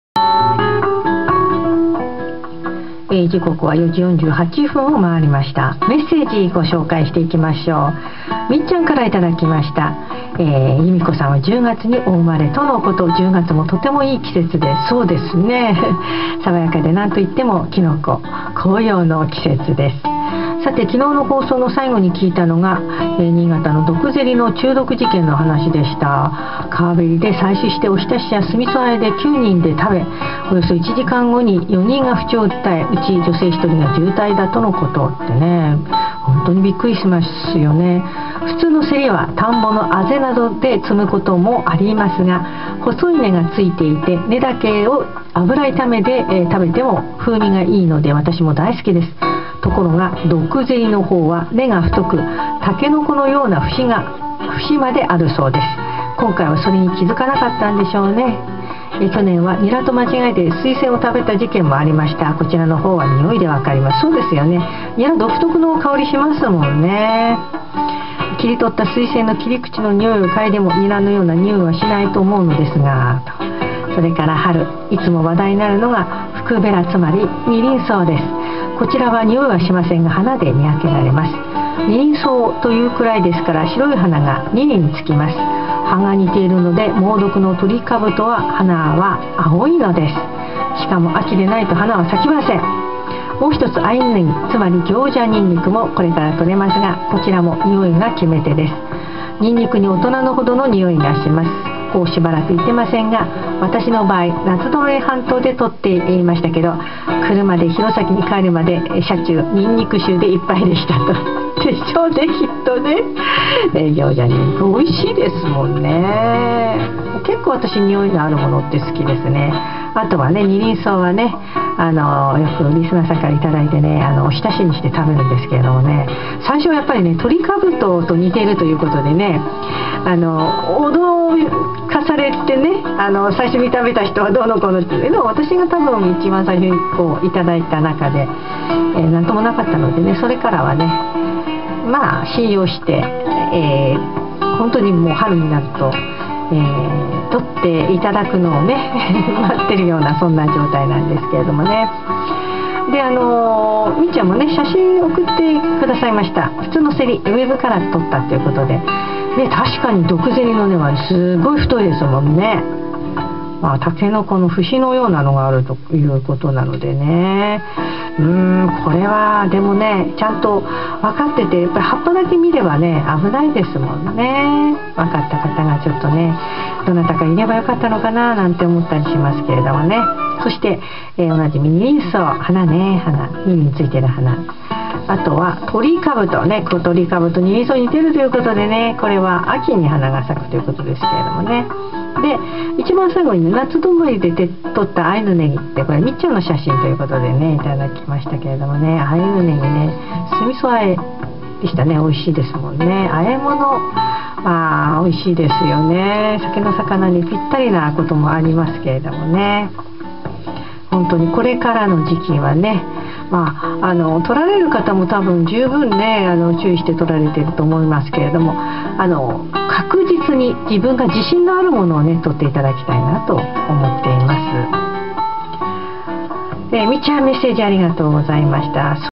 ドクゼリ中毒
(新潟市保健所)
「ドクゼリ」を誤食した市内の男女４人が、
中毒を起こしたと発表。 2013/04/01

内、女性１人は意識不明の重体らしい。
(先月３１日、知人が新発田市内の川べりで採取した物)